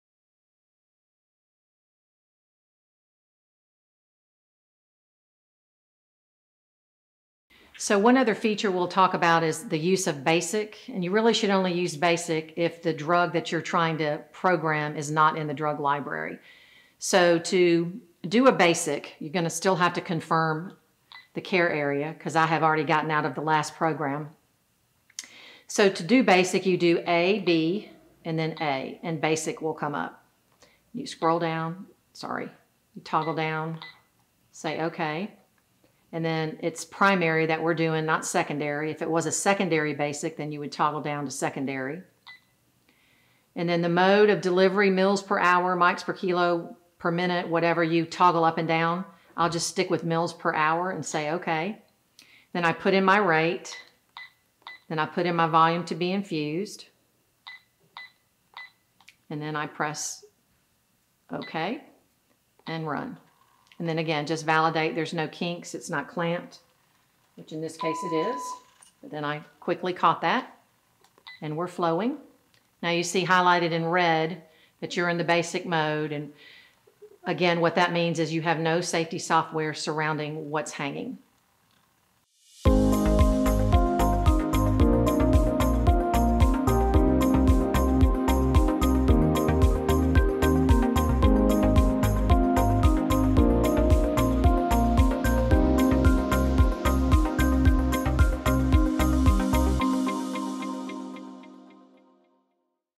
How To Program a Basic Infusion on the Sigma Spectrum IV Pump
This video shows how to program a basic IV infusion on the Sigma Spectrum infusion pump without using the drug library.

Be sure to check out the full training video for the Sigma Spectrum IV pump here: